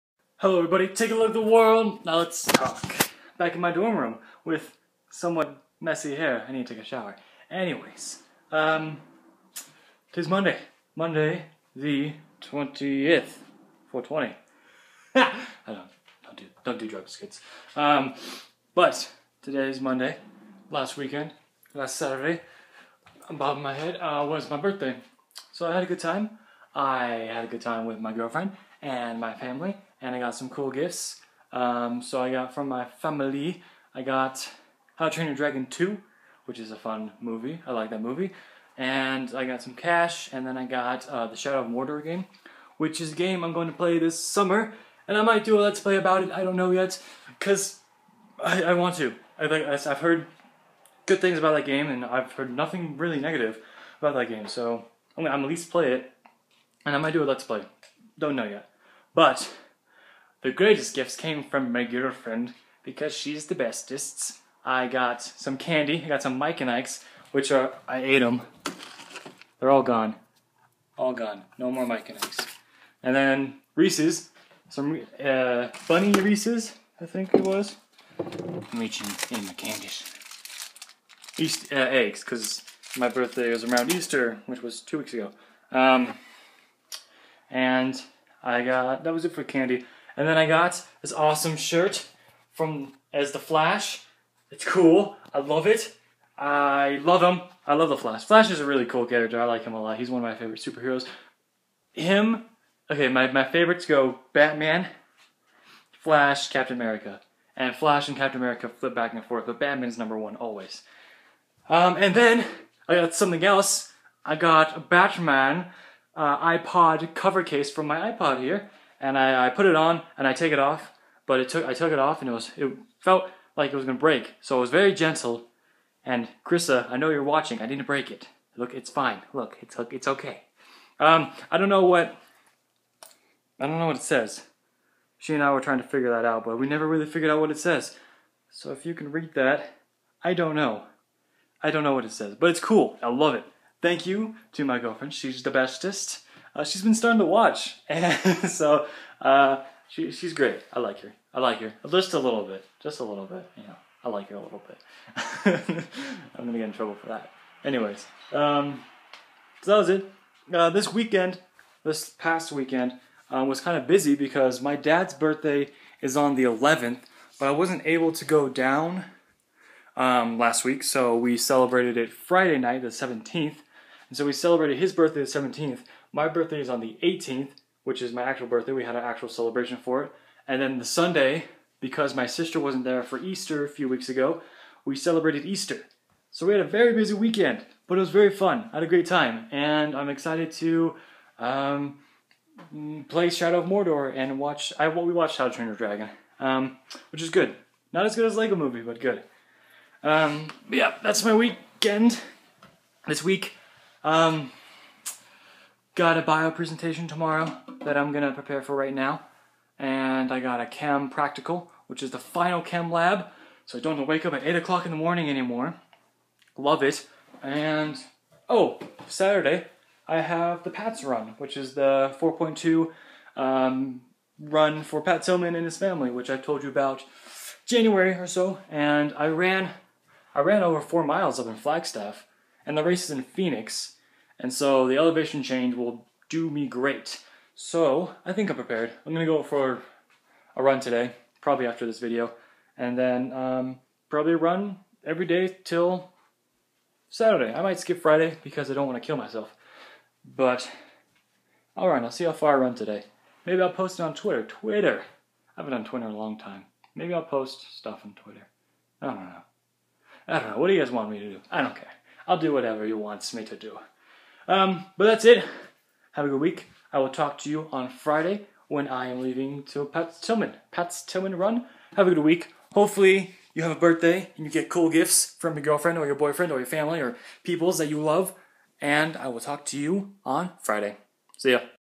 I had a birthday!
Today i talk about my past weekend and my birthday!!